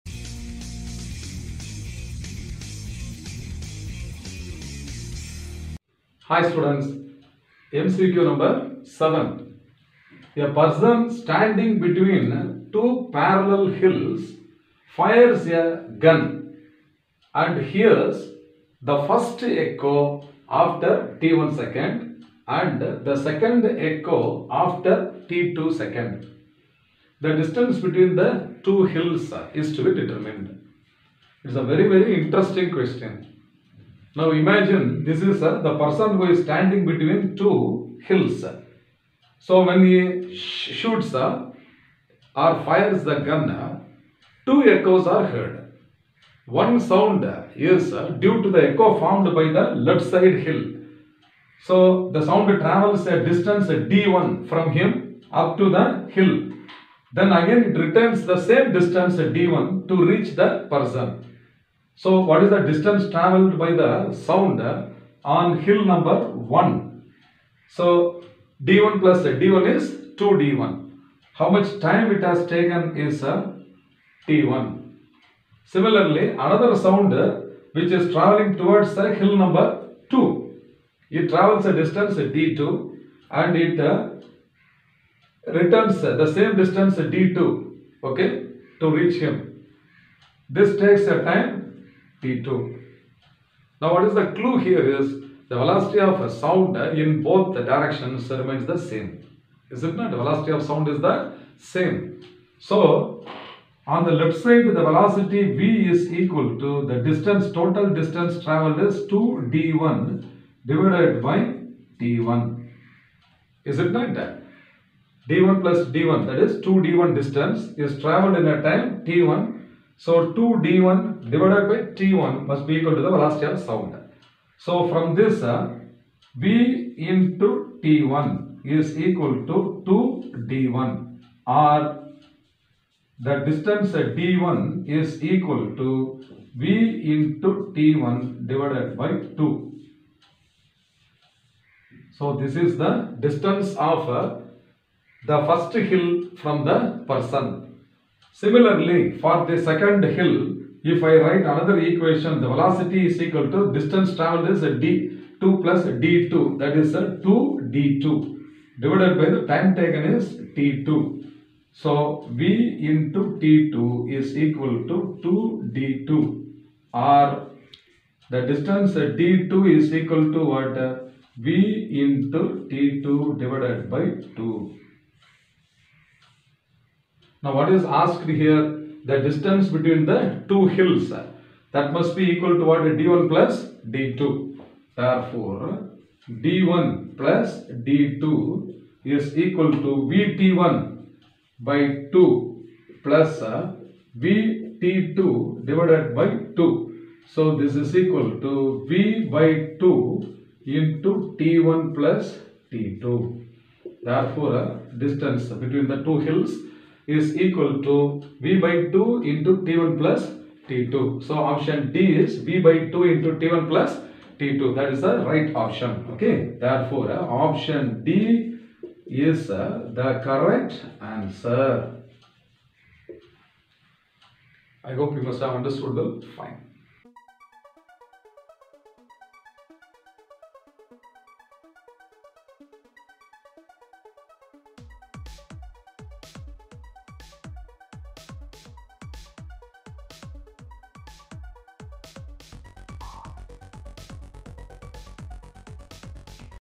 MCQ 7 with solution | WAVES | Class 11 | Samacheer Kalvi | TN Stateboard| Physics is everywhere
Physics Lectures by P.SARAVANAN

Question 7.
A person standing between two parallel hills fires a gun and hears the first echo after t1 sec and the second echo after t2 sec. The distance between the two hills is :

The solution to MCQs in Waves

Solution to Numerical Problems in Waves